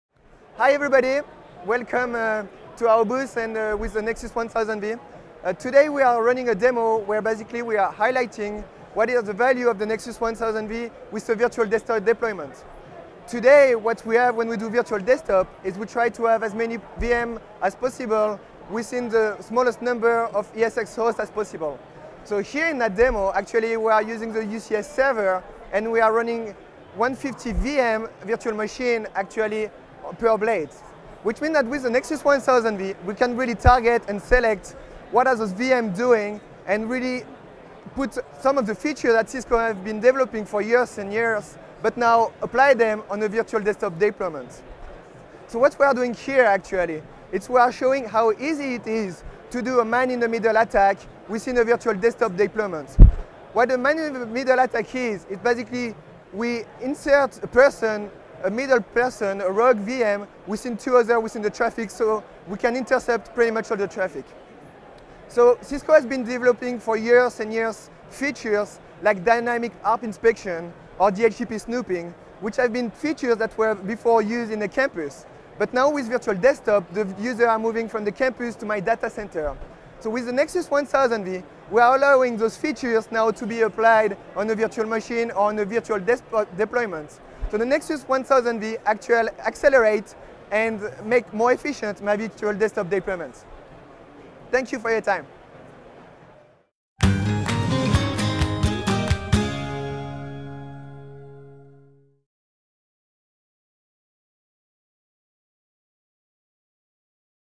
Virtual Desktop deployment with Nexus 1000V
In this video, Pierre-Emmanuel Ettori, Engineer, Cisco Server Access and Virtualization Business Unit explains the contribution of the Cisco nexus 1000V to the virtual desktop deployment.